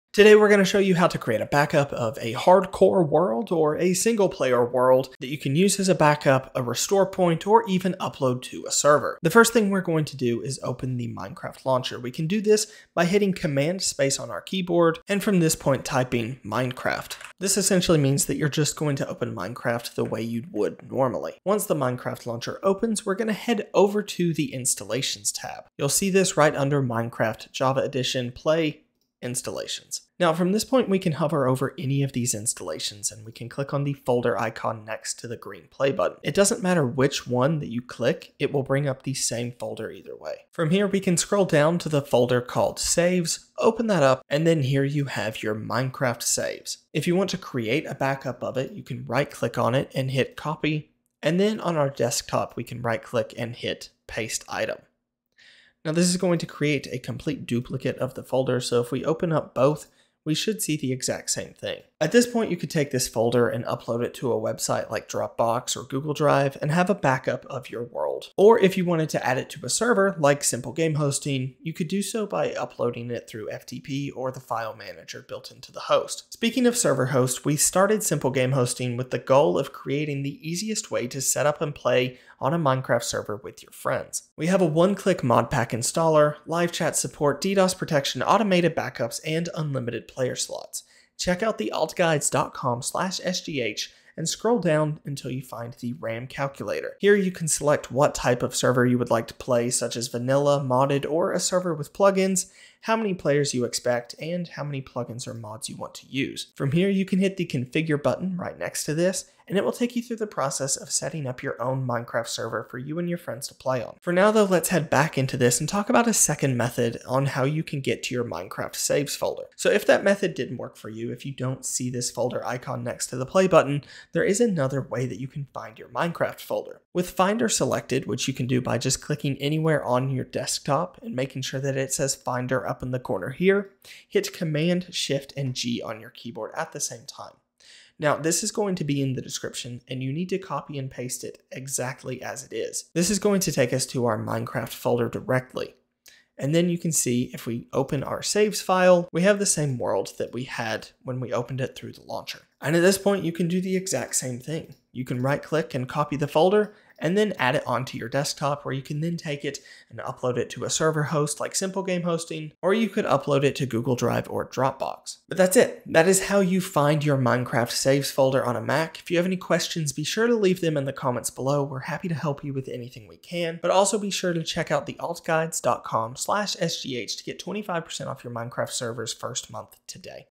How to Find Your Minecraft Saves & Worlds (Mac)
Knowing how to find your Minecraft Saves & Worlds on a Mac can save you loads of time if you’re wanting to create a backup of your world, send it to a friend, or upload your Minecraft world to a server. Today, I’ll show you how to find the Minecraft Saves folder on a Mac so that you can access your world files.

Finder Go Line:
~/Users/patrick/Library/Application Support/minecraft

About this video:

Accessing the Minecraft Saves folder on a Mac is extremely easy and only requires a few steps to open it. Even better, there are two very easy methods you can use to find the Minecraft Saves and Worlds folder on a Mac. Today, I’m going to show you both of ways that you can open the Minecraft world folder on your Mac. With the Minecraft saves/world folder, you can create a copy for a backup (including a hardcore world), send it to a friend, or even upload it to a Minecraft server host like SimpleGameHosting.

Let’s dive right in to how to find the Minecraft Saves folder on a Mac!

Method 1: Minecraft Launcher

The first method you can use to find the Minecraft Saves folder on a Mac is through the Minecraft launcher. To do this, open Minecraft as you normally would on your Mac. When the Minecraft launcher on your Mac opens, head over to the installations tab found at the top of the screen. This will show you all of the active Minecraft installations that you have on your Mac, such as Vanilla versions, modded versions, and more. At this point, hover over any of the Minecraft installations on the list. You should see a green button with the text ‘Play’ pop up to the right of each installation as you hover over it. To the right of that, you’ll see a folder icon in grey. Press this.

This will open your Minecraft folder - the root Minecraft folder on your Mac. Here, you can access all of your Minecraft files, such as resource packs, mods, texture packs, and most importantly - saves. This folder contains every important part of Minecraft, so be sure not to mess with anything unless you know what you are doing. Removing something unintentionally can break your game.

From here, you should see a folder called ‘Saves.’ This is where all of your Minecraft saves and worlds are located. From here, find the folder with the same name as the world you want to either backup, upload, or make a copy of. Right click, hit ‘Copy’ and then you can close out of the Minecraft Saves folder in Finder.

Now, on your Desktop, right click, and hit ‘Paste.’ This will create a copy of the Minecraft World folder or Minecraft save that you want to create a backup of or send to a friend or upload to a server. Now, you have a backup of your Minecraft world folder and a backup of your Minecraft save.

Method 2: Finder → Go

If the first method doesn’t work for you, there is another way that you can find the Minecraft saves and worlds folder on a Mac. Click on your desktop and ensure that the menu bar says ‘Finder’ in the top left corner of the screen. From here, hit these three keys at the same time on your Mac keyboard - CMD + Shift + G. This will open the ‘Go’ window where you will need to paste this exact line without any changes.

~/Users/patrick/Library/Application Support/minecraft

Once you have pasted that into the ‘Go’ window, press Return/Enter on your keyboard. Now, you’ll be taken directly to the Minecraft folder on your Mac. From here, scroll until you see the ‘Saves’ folder, which contains all of your Minecraft worlds and saves.

With that open, you can follow the same steps as above. Right click on the folder or world that you want to create a copy/backup of and press ‘Copy.’ From here, go to your Desktop or any other folder on your Mac and hit CMD + V on your keyboard. This will paste the Minecraft save right there. Likewise, if you don’t want to move it anywhere else, you can always paste a second version in the world right in the Minecraft Saves Folder. Rename it to something else like ‘world-backup,’ and press enter. This will create a backup of your Minecraft world, which you can also use to upload to a service like Dropbox, Google Drive, or a Minecraft Server Hosting company like SimpleGameHosting.

If you enjoyed this video on finding the Minecraft worlds folder on a Mac or if this video helped you find the Minecraft saves folder on macOS, be sure to leave a comment below.